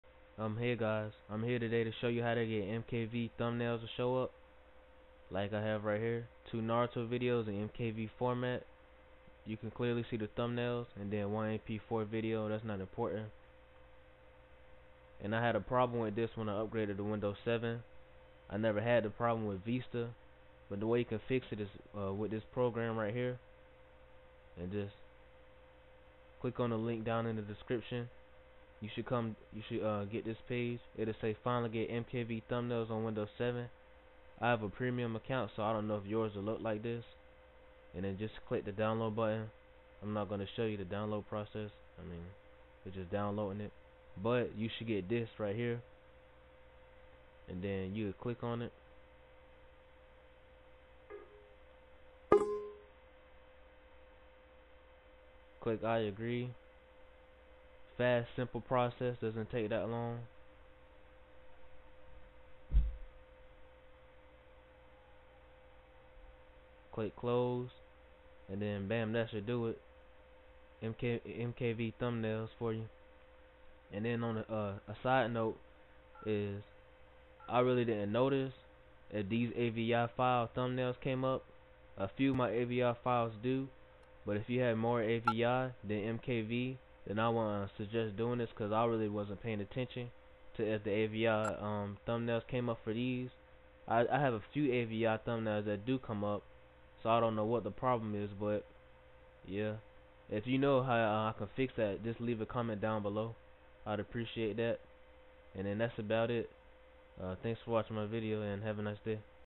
How to get MKV thumbnails (windows 7)
Sorry the link is down because of megaupload so I put up a new link for everyone.

Note: The downside to this that I discovered is that if you have a lot of MKV videos then when you go to certain folders with certain MKV videos it will restart windows explore constantly and you will have to uninstall this program in safe mode.  But I have over 150 MKV videos so something was going to happen eventually.